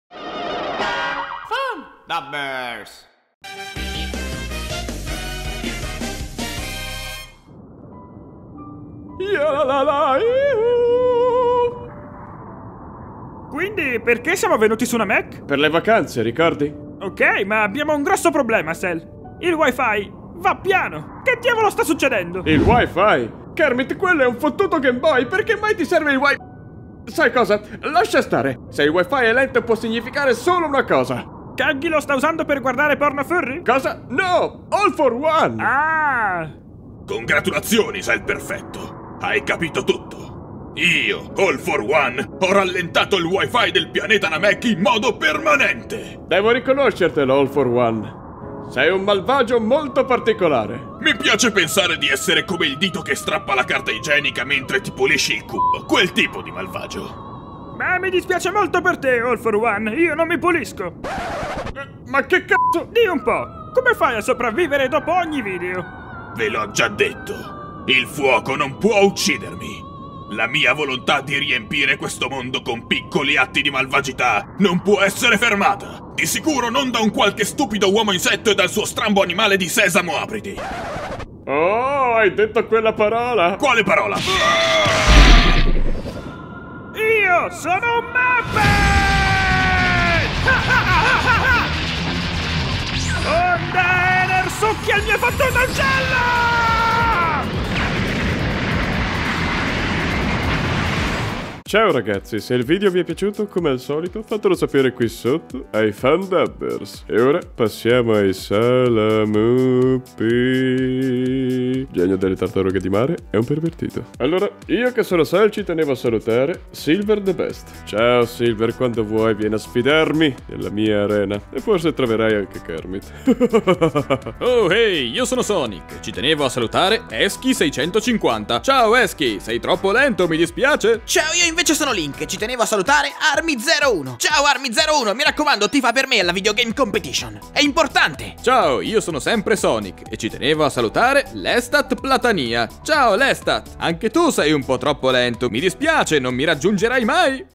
Perfect Cell Vs All For One (Parte 3) | DEVILARTEMIS - DOPPIAGGIO ITA
Aaaaah non c'è niente di meglio di una bella vacanza sul pianeta Nameck... Cosa potrebbe mai andare storto? 😁😁😁
▼▼▼▼▼ CONTINUA SOTTO ▼▼▼▼▼

Microfono Condensatore USB Yeti
Videocamera Xiaomi Yi
Filtro Anti Pop per microfono

║INFORMAZIONI SOCIAL!  ║

║GUARDA ALTRI VIDEO!  ║